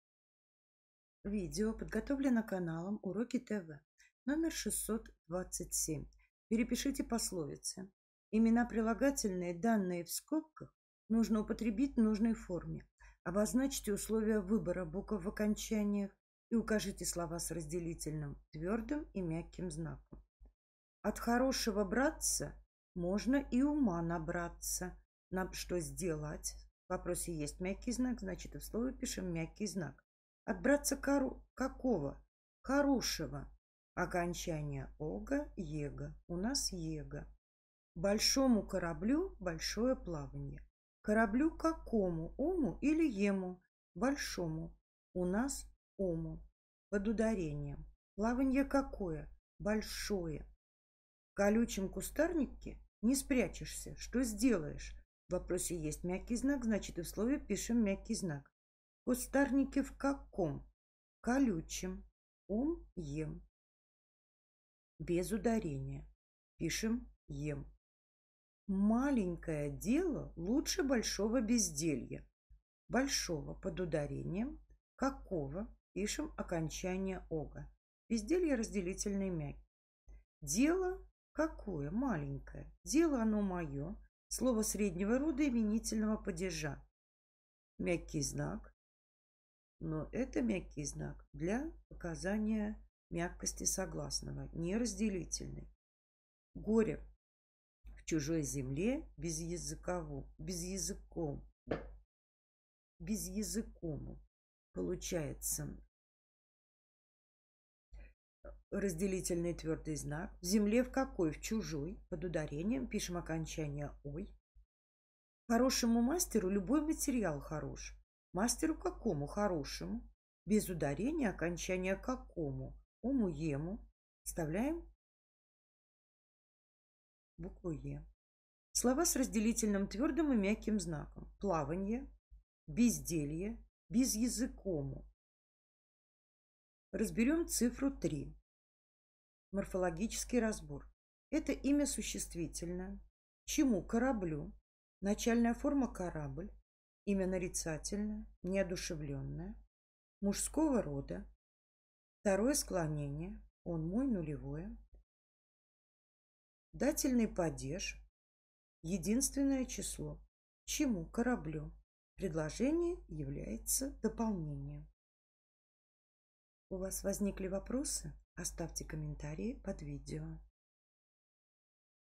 Упражнение №627 — Гдз по русскому языку 5 класс (Ладыженская) 2019 часть 2
В данном номере переписываем пословицы. Имена прилагательные,  данные в скобках  употребляем в нужной форме. Обозначаем условия выбора букв в окончаниях. Указываем слова с разделительными ь и ь.